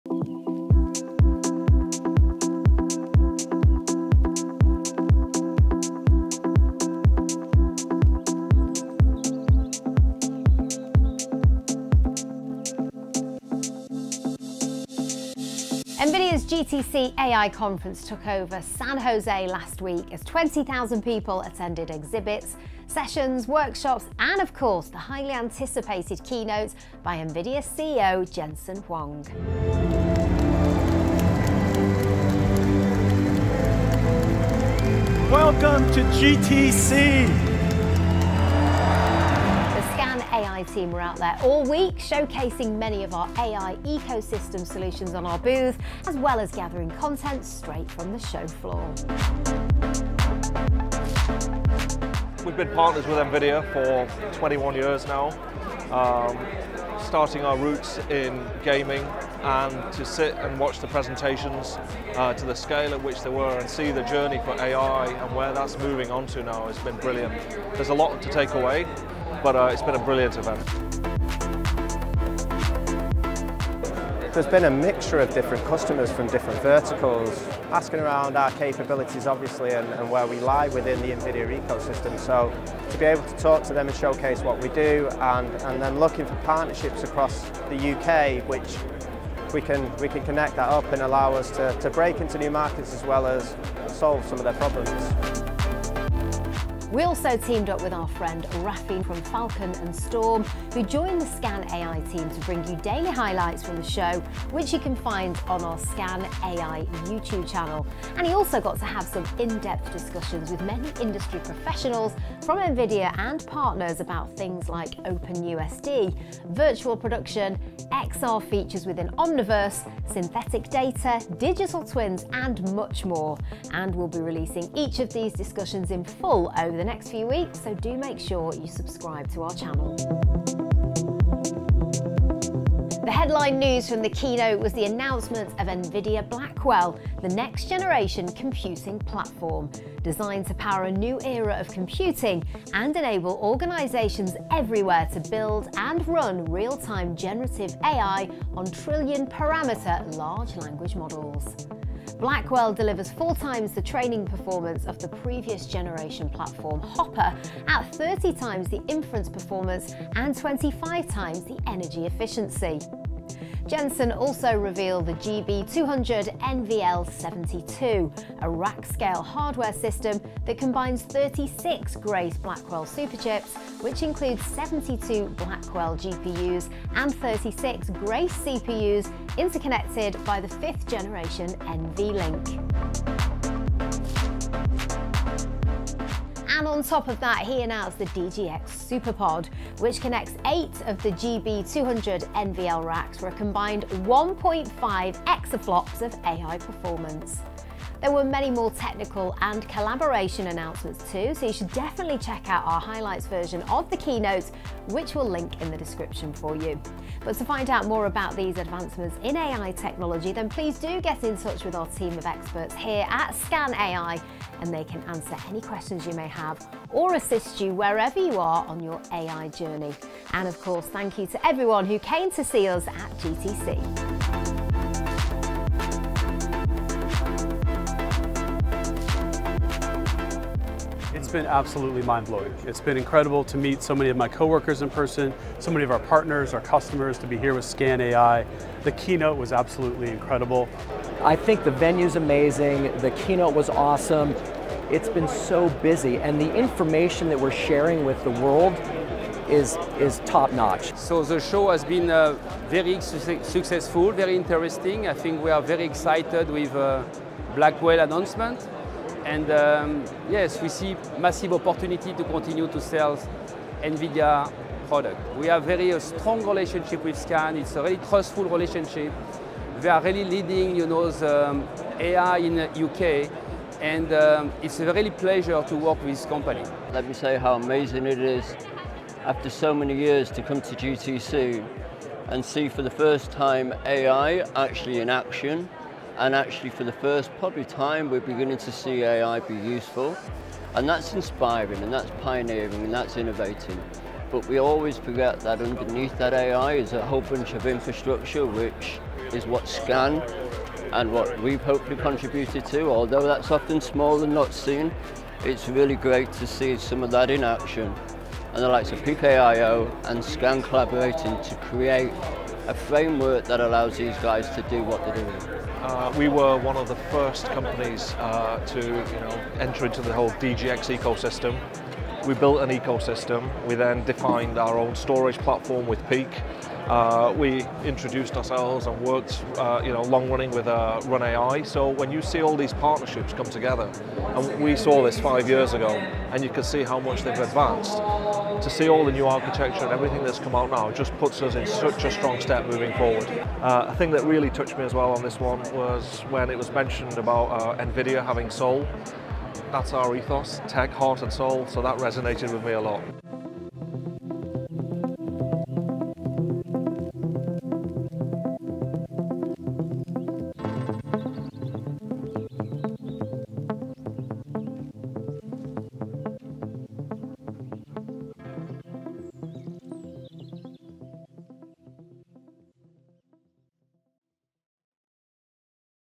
NVIDIA GTC 2024 Round Up - Scan AI
In today's video we do a complete round of of NVIDIA GTC 2024! From the Scan AI stand, CEO Jensen Huang's Keynote, to all of the experts we interviewed - we cover it all!